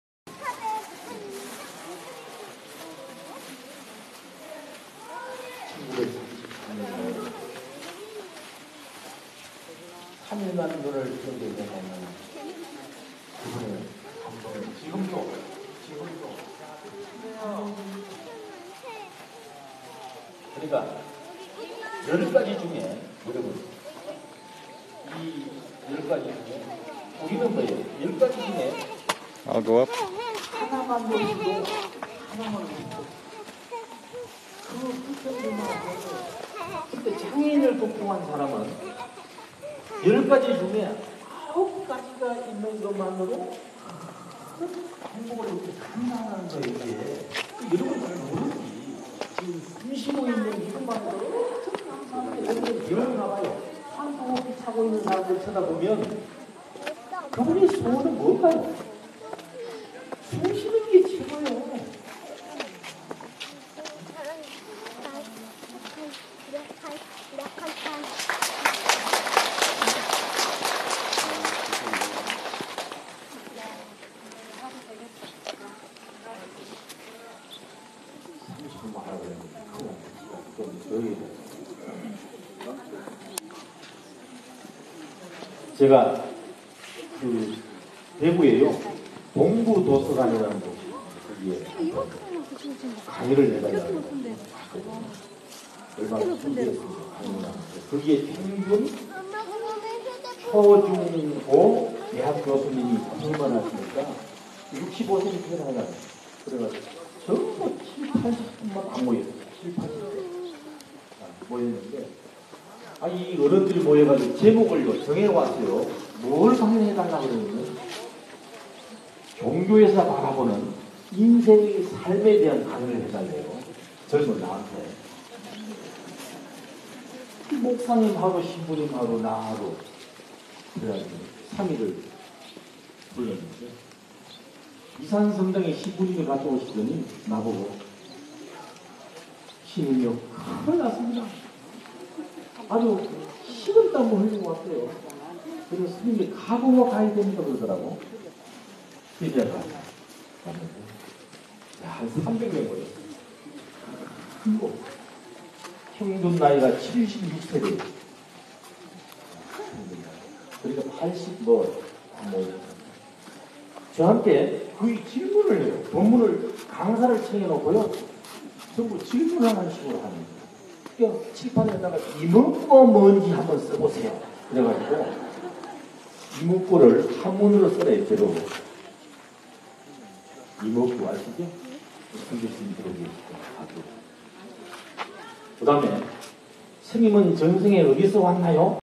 Courtyard and Main Shrine at Haeinsa Temple in South Korea
Climbing up from a courtyard to the main shrine and taking in part of a lecture by a local monk.